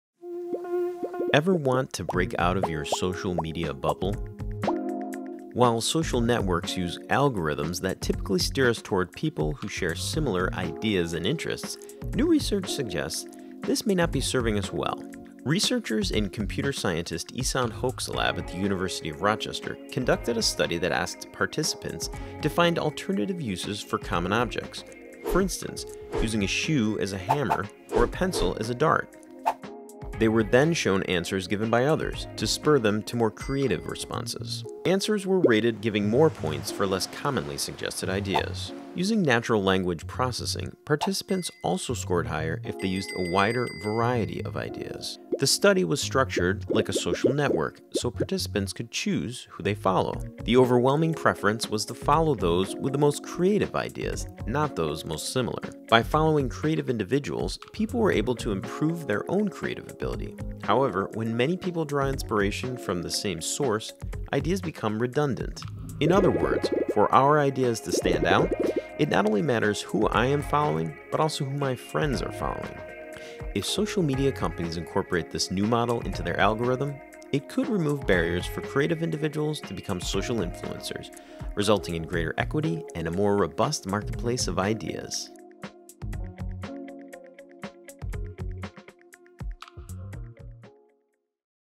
Building More Creative Social Networks
The algorithms that social media platforms use to recommend who we should “follow” are designed to steer us to people who likely share the same ideas and interests.

However, platforms like Facebook, Twitter, and Instagram could help us become more creative – a quality that is increasingly valued in our society—if they instead steered us to people with ideas and interests different from our own, say researchers at the University of Rochester.

In a new paper in the Journal of the Royal Society Interface, they describe conducting a novel experiment in which participants were asked to think of unusual, “creative” uses for commonplace items – using a pencil as a dart, for example, or a shoe as a hammer. They were also given the opportunity to view the ideas of other participants and then decide which of them they would like to “follow” for creative inspiration.

“We found that the participants overwhelmingly chose to follow peers who had the most creative ideas,” says lead author Raiyan Abdul Baten, a PhD student in the human-computer interaction lab of Ehsan Hoque, an associate professor of computer science.

“We confirmed that following highly creative peers indeed helped people generate novel ideas themselves—the intuition being, if you bump up against out-of-the-box ideas, chances are higher that you will be able to combine your own ideas with ideas you didn’t originally think of. Such creative recombinations can lead to further novel ideas.”

However, the researchers also found that when many people share the same sources of inspiration, even their own independently generated ideas can become similar to each other. The finding suggests potential drawbacks if too many people follow a relatively small number of highly creative idea leaders, resulting in a decrease in the overall diversity of thinking across the network. The “thought leaders” themselves need to keep generating outlier ideas in order to “stay relevant and remain ahead of others.”

Members of Hoque’s team designed their experiment using a blend of techniques and findings from a variety of fields, including collective intelligence, computer science, the psychology of creativity, and network design. Their findings bridge a “huge research gap,” Baten says, documenting for the first time the network-level dynamics that occur when people are presented with divergent ideas in a web-based setting.

In order to replicate those dynamics, participants recruited from Amazon Mechanical Turk were divided into two sets of “nodes” in a simulated social network. Participants assigned to nodes as “alters” independently came up with ideas for alternative uses for commonplace objects. Participants placed in nodes as “egos” were each connected to two of the “alters.” After spending three minutes generating their own ideas, the “egos” were then allowed to see the ideas of their “alters” and were given three additional minutes to list further ideas.

The “egos” were then asked to rate the novelty of the ideas suggested by all of the “alters” and, finally, to choose whether to continue following the two “alters” they were initially assigned or instead follow others.

This process was repeated five rounds, each time the participants coming up with alternative uses for a different commonplace object.

The relative “creativity” of the ideas was rated in three ways:

1. By other study participants who were not assigned to any of the nodes
2. Giving more points to ideas less frequently suggested
3. By using natural language processing to help determine which participants had a wider variety of ideas, and therefore were more creative

The researchers will continue to extend the scope of the experiment, examining for example, how the gender and race of participants may influence outcomes.

Gourab Ghoshal, associate professor of physics, mathematics and computer science at Rochester who was also part of the work.

“There has been a lot of work on the optimal topologies (putting people together in the right way) to enhance team performance,” he says. “This, however, has been treated from a static context. In reality, social networks are constantly evolving, and the counterintuitive results presented here, suggests the situation is far more complex. This has profound implications in how people interact on social media, or indeed, work together in teams.”

Other authors include James Bagrow, associate professor of mathematics and statistics at the University of Vermont; and Daryl Bagley, a PhD student, and Ashely Tenesaca and Famous Clark, undergraduate researchers, in Hoque’s lab.

The project was supported with funding from the National Science Foundation.

Be sure to like the University of Rochester on our Facebook